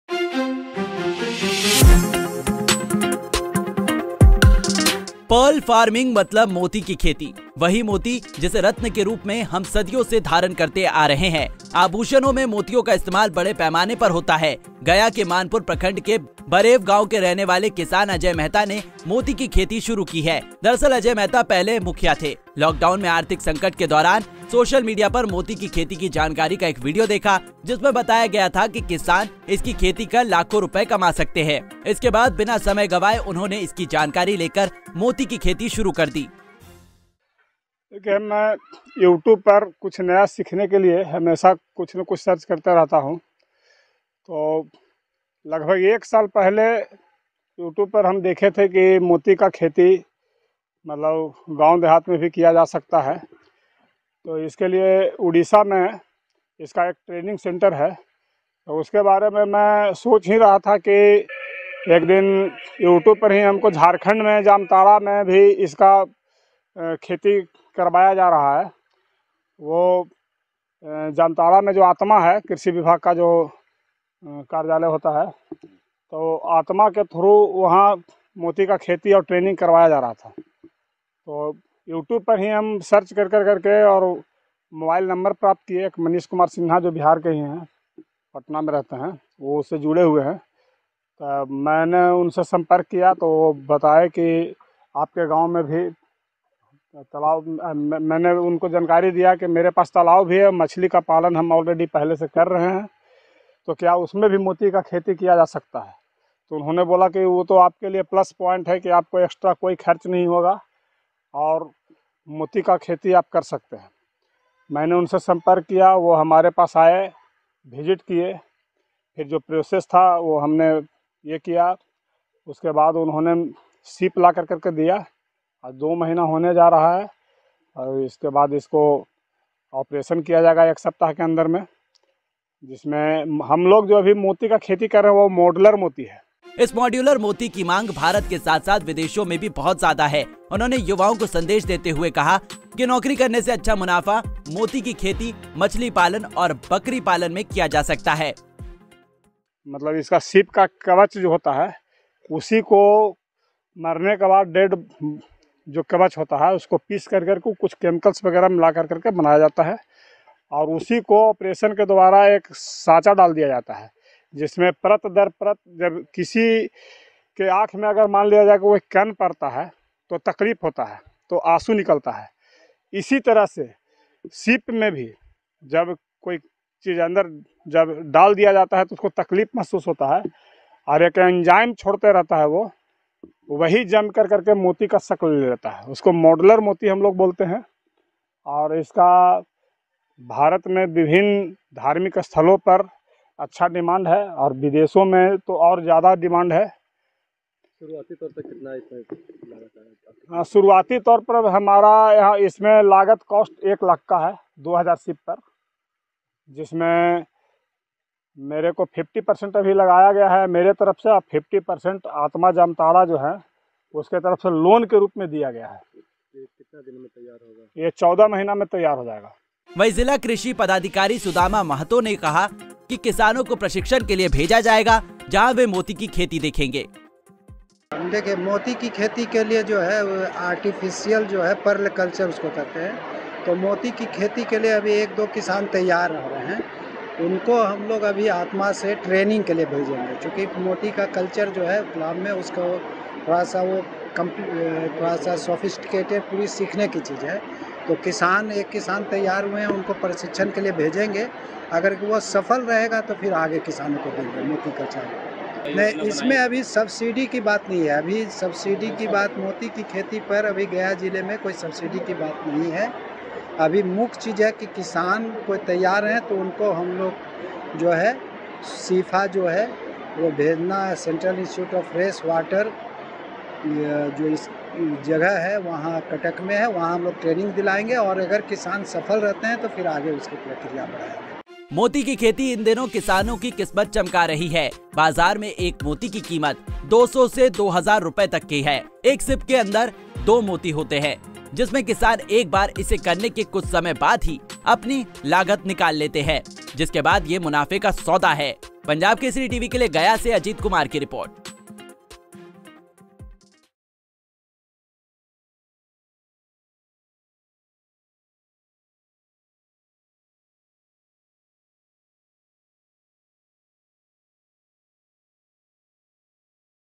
मोती की खेती से चमकेगी किस्मत:सोशल मीडिया पर सीखा Pearl Farming, अब हो रहा तगड़ा मुनाफा
पर्ल फार्मिंग(Pearl Farming) मतलब मोती की खेती...वहीं मोती, जिसे रत्न के रूप में हम सदियों से धारण करते रहे हैं...आभूषणों में मोतियों का इस्तेमाल बड़े पैमाने पर होता है...

Pearl farming means the cultivation of pearls, whereas pearls, which we have been wearing as gems for centuries, are used extensively in jewelry.